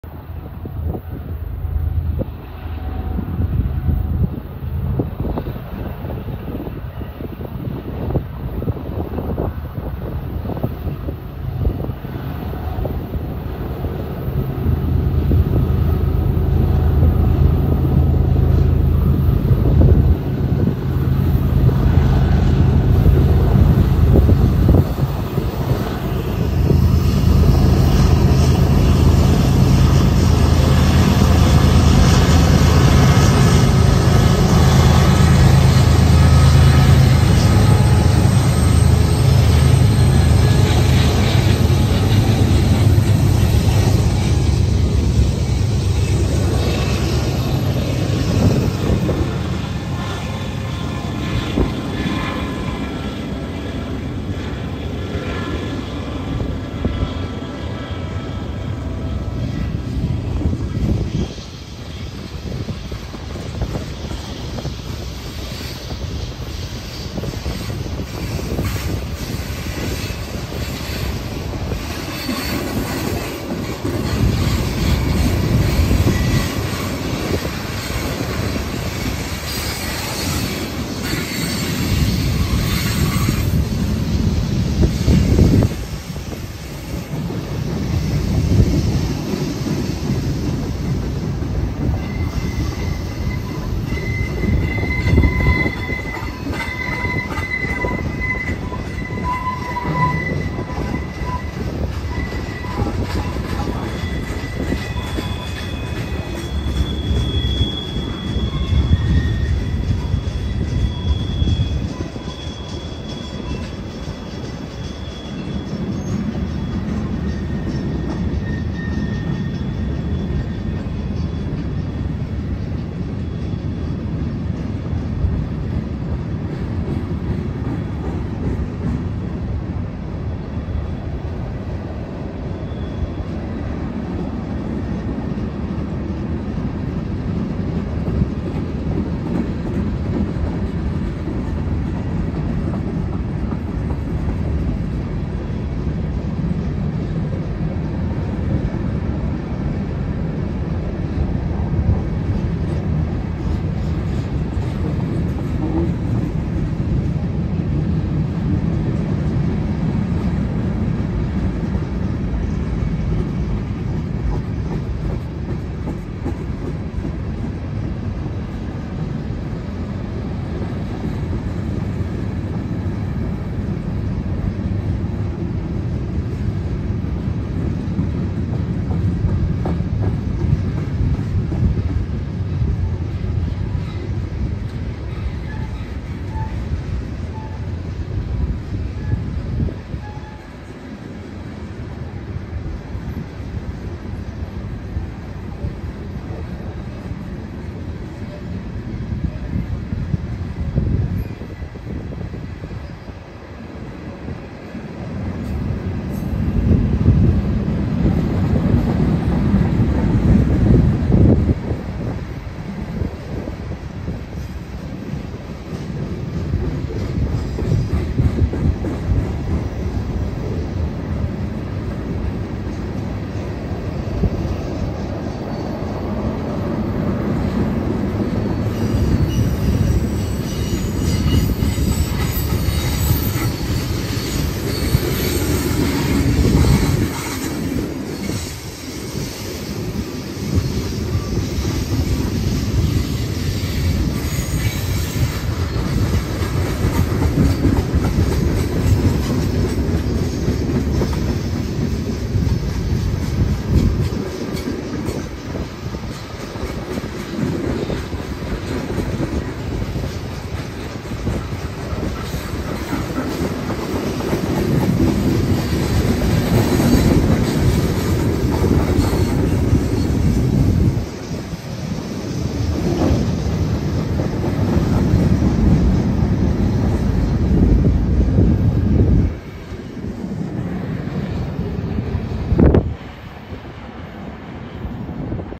GP60M-3 & KCS on the H-DENAMS Led by a GEVO with K5LLA Horn Rounds the Bend in Palmer Lake, CO
BNSF #7335
KCS #4842
BNSF #5051
BNSF #103